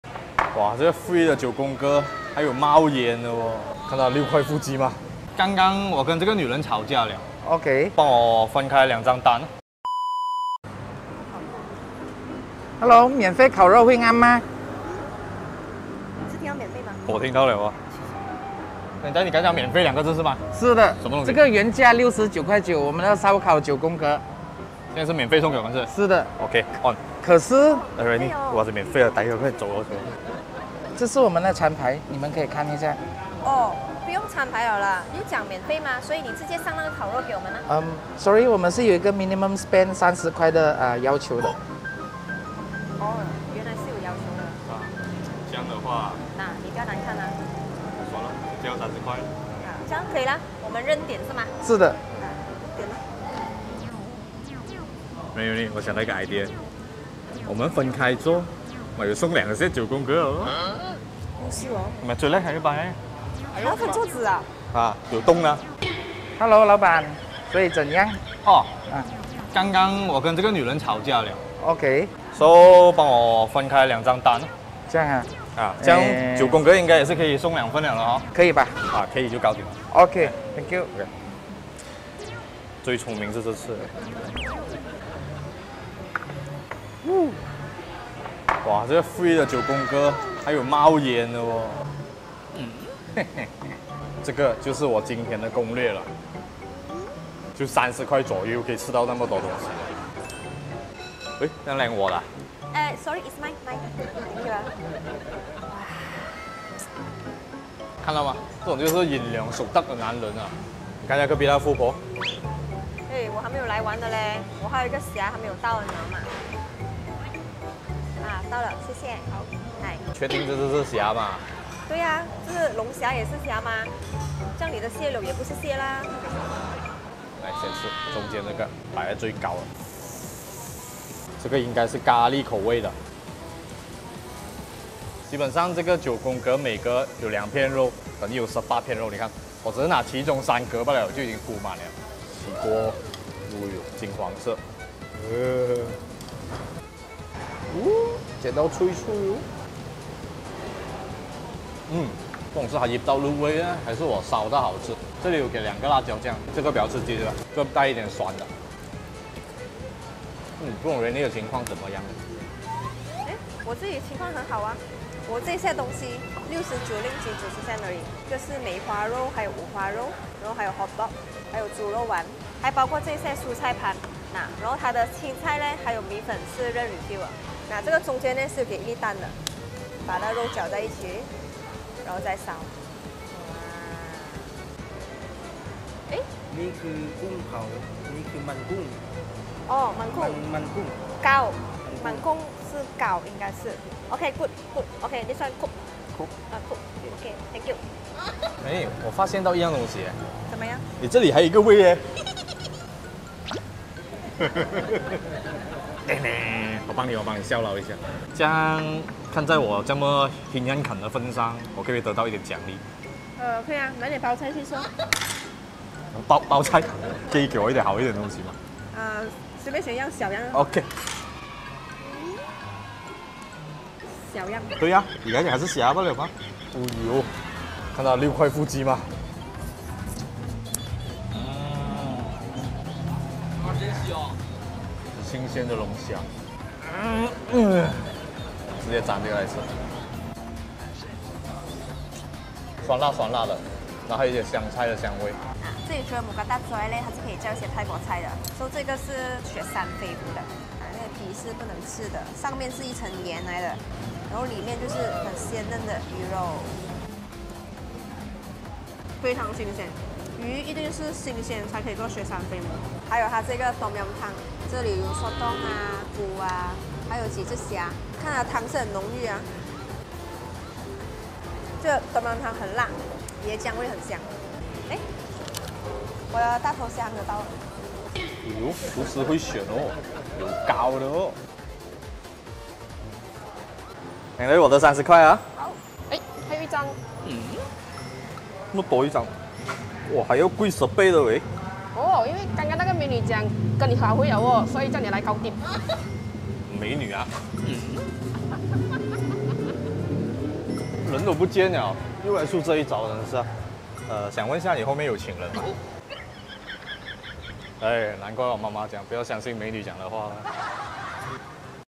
今天去吃KL新开的泰式烧烤😋只需要吃上RM30，就送九宫格烤肉🤩！
新开张大大间泰式 Mookata🔥！只需吃满30块，就免费送 九宫格套餐，一次过吃九种口味的肉肉🥓！还可以叫泰国菜吃饭、吃泰式烤海鲜😍！ 每天营业至半夜5am！
看着朋友圈都在晒去泰国吃吃喝喝的照片，自己却没时间去🥺？小编这就推荐你们一家新开的泰国餐厅🇹🇭！这家位于📍Pandan Indah的【Aroy Mookata】主打泰式烧烤🔥、各种各样的配料、到经典泰国料理👍🏻！这里通通都吃到😋！一班朋友一起聚餐最合适，重点有吃还有送！✌️
这里有多种Mookata套餐供你选择，也可以单点配料👌🏻！目前有超值优惠，只需要吃上RM30，就免费送🆓《招牌九宫格》*原价RM69.90，简直泰值得啦😍！里面包括有九种自家腌制的口味，黑胡椒、tomyam、泰式咖喱、香茅等，每种口味都有两大片✌️，不会因为是免费的而偷工减料哦🤤！肉食爱好者必点《精选猪肉套餐》🐽，里面包括有梅花肉、五花肉、香肠、猪肉丸，新鲜的肉肉先沾蛋液再拿去烤至金黄色，配上灵魂辣椒酱🌶️，一大口送入口中，简直太享受啦😋！只要点套餐还可以免费refill蔬菜和冬粉哦👏🏻！
还觉得不够丰富🤔？那就点几份正宗泰式美食，一次过满足你的食欲😜！推荐的有《Tomyam Prawns》🦐，汤汁浓郁酸辣、椰香十足，超开胃！还有肉质鲜嫩的《雪山飞狐》🐟、以及各种烧烤海鲜像是《烤大头虾》、《烤龙虾🦞》等！特别一提，这里的海鲜都是新鲜游水的呢，海鲜控千万别错过😉！
这里除了能吃到泰式美食和烧烤，用餐环境也宽敞舒适、加上高高的天花板，吃起烧烤来也不会觉得闷热🥰！还有他们每天营业至凌晨5am，大半夜想吃泰国餐，直接来这里就对了😉！
【Aroy Mookata】
📍: 1, Jalan Pandan Indah 4/34, Pandan Indah, 55100 Kuala Lumpur, Wilayah Persekutuan Kuala Lumpur.

主持人
Rainy Loke Li Yin
Wilson Lau